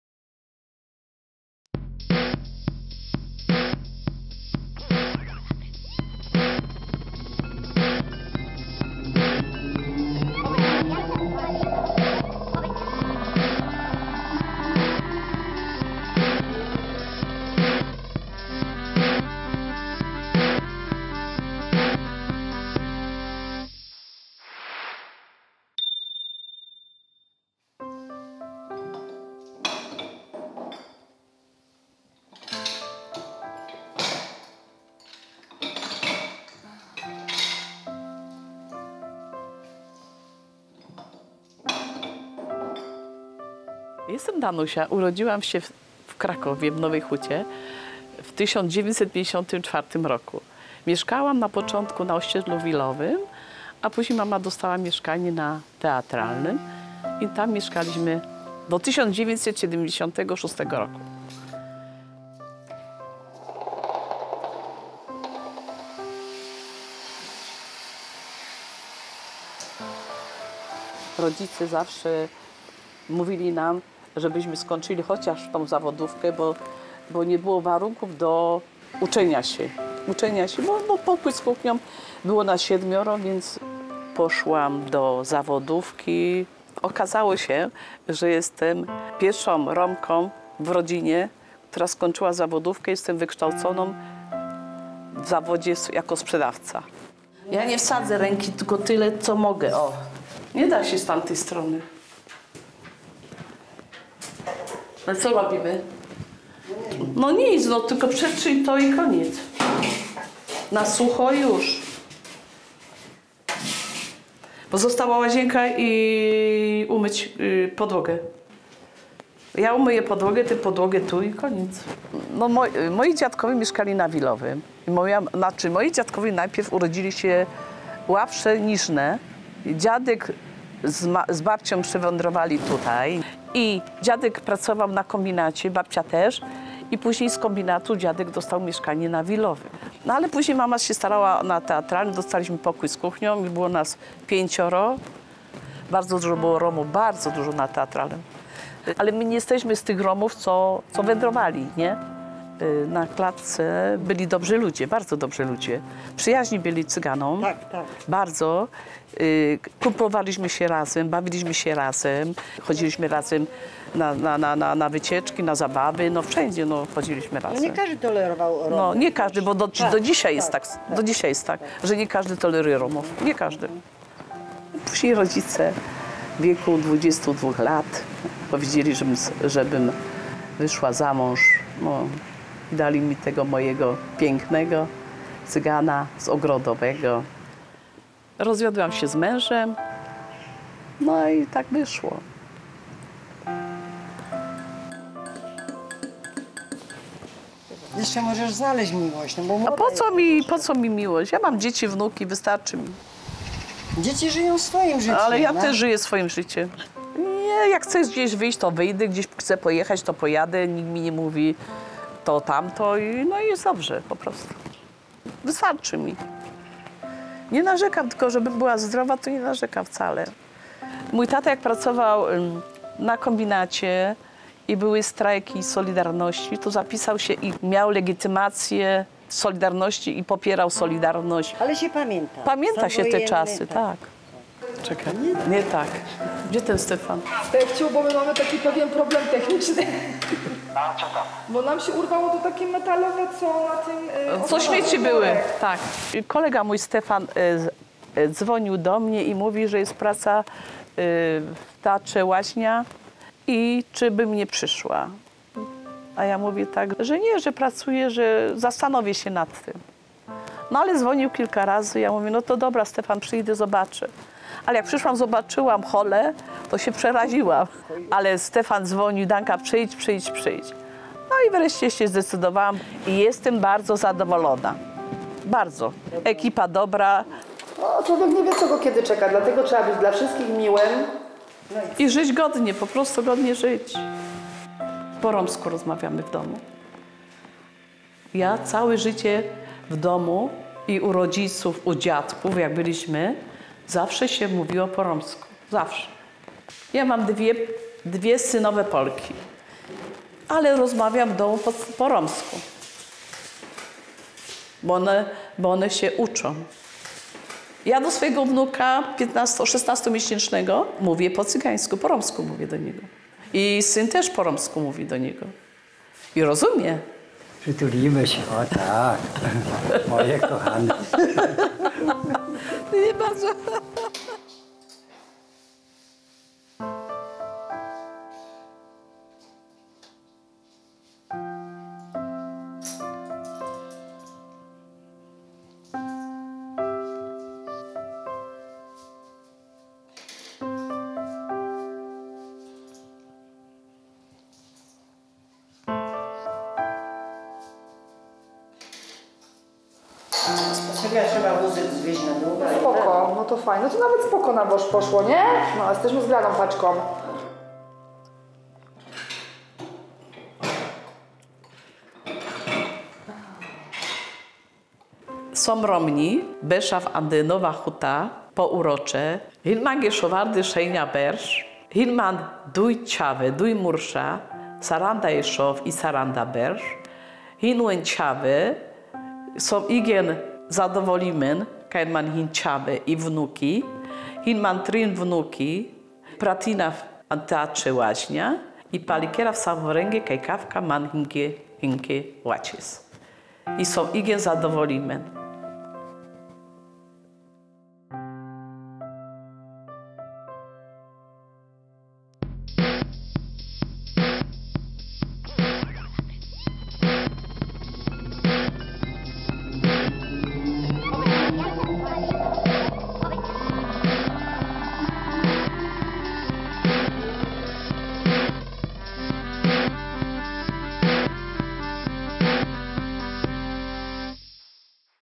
Nowohucka Kronika Filmowa nr 756
Danuta Mirga urodziła się w 1954 roku i jest Romką. Należy do pierwszego pokolenia dzieci urodzonych tuż po powstaniu Nowej Huty i do dziś tutaj mieszka. Ma dwójkę synów. Dziadek i babcia Pani Danusi pochodzą z Łapsze Niżne i stamtąd przywędrowali do Nowej Huty pracować na kombinacie. Tata Danusi też pracował na kombinacie i należał do Solidarności. Pani Danusia jako dziecko nie odczuwała wykluczenia z powodu tego, że jest Romką. Początki asymilacji Romów w Nowej Hucie wspomina pozytywnie. Jako pierwsza w rodzinie zdobyła wykształcenie – skończyła zespół szkół spożywczych z tytułem sprzedawcy. Obecnie pracuje jako sprzątaczka w Teatrze Łaźnia Nowa - nie narzeka, w pracy ma grupę fajnych koleżanek, lubi z nimi pracować. Do dziś posługuje się językiem romskim, którego uczy swoich wnuków.